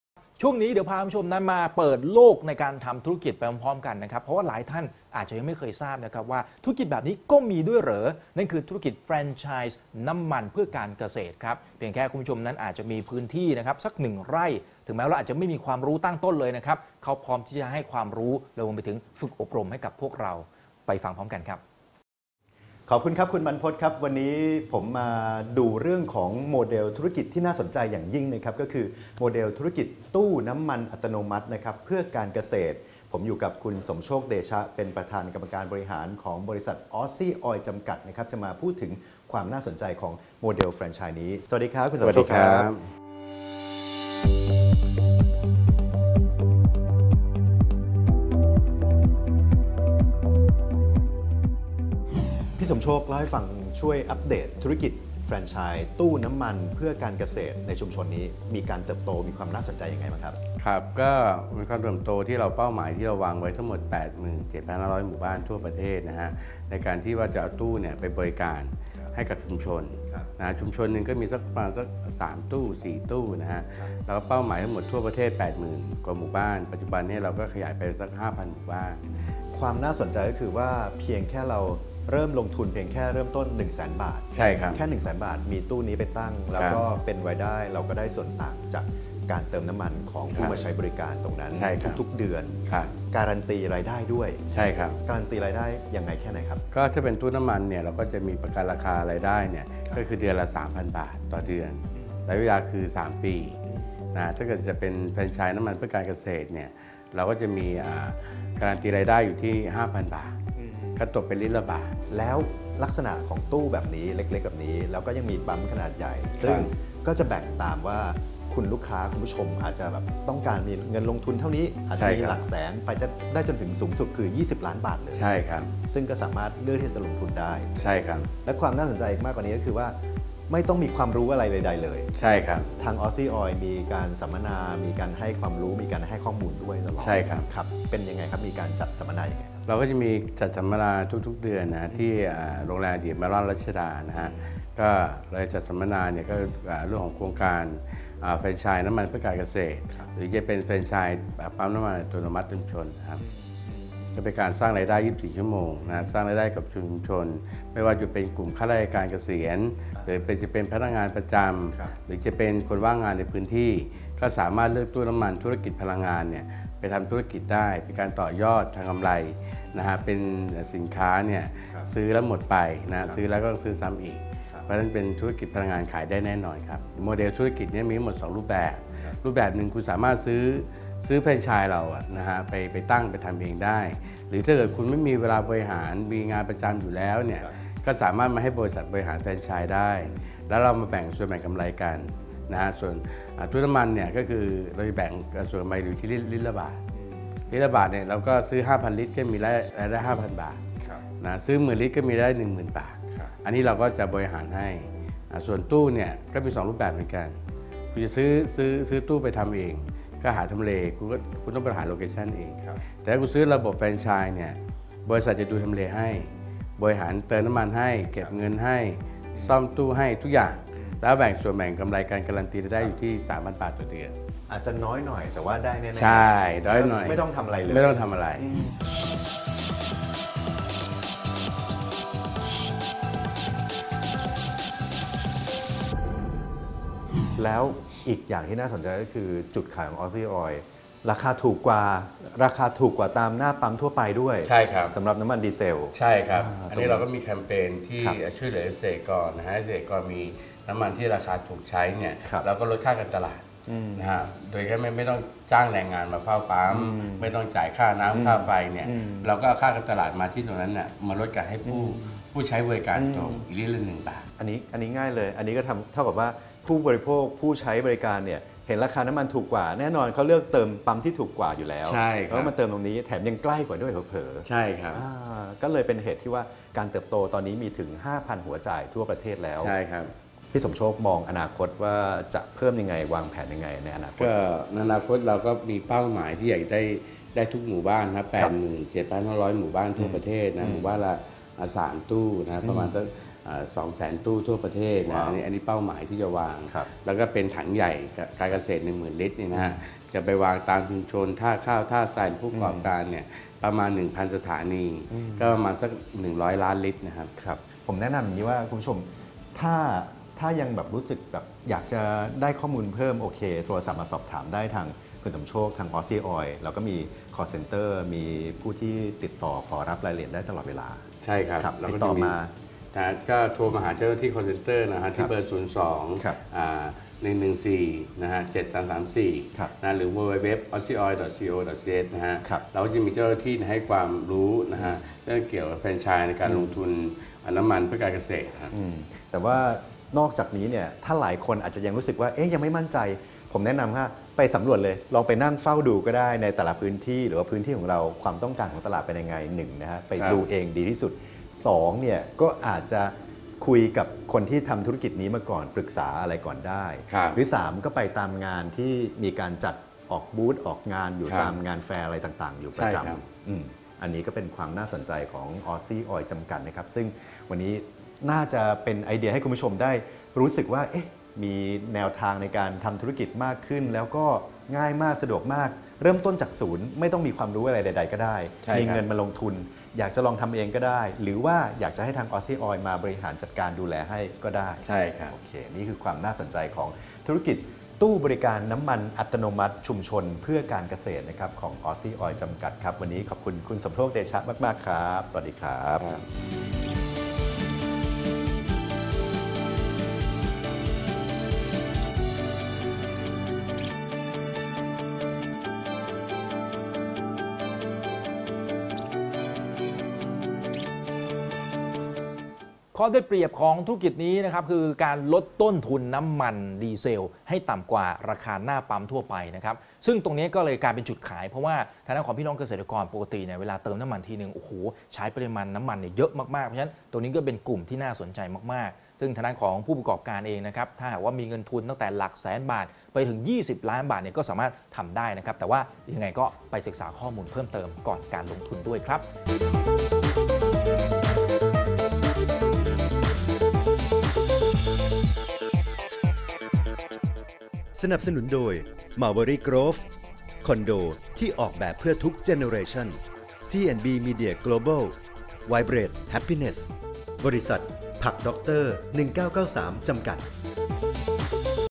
โมเดลพารวย แฟรนไชส์น้ำมันเพื่อการเกษตร
โมเดลพารวย แฟรนไชส์น้ำมันเพื่อการเกษตร จากบริษัทออสซี่ออยล์ จำกัด ธุรกิจแฟรนไชส์ปั๊มน้ำมันหัวจ่ายให้เช่า หากมีพื้นที่เล็กๆ ต่างจังหวัดก็สามารถหารายได้ ได้ง่ายๆ รายใหญ่ก็ทำได้เช่นกัน เป็นโอกาสน่าลงทุน ที่แม้ไม่มีความรู้เลย ก็มีอบรมให้ตั้งแต่เริ่มต้น

ติดต่อบริษัท ออสซี่ออยล์ จำกัด โทร.02-114-7334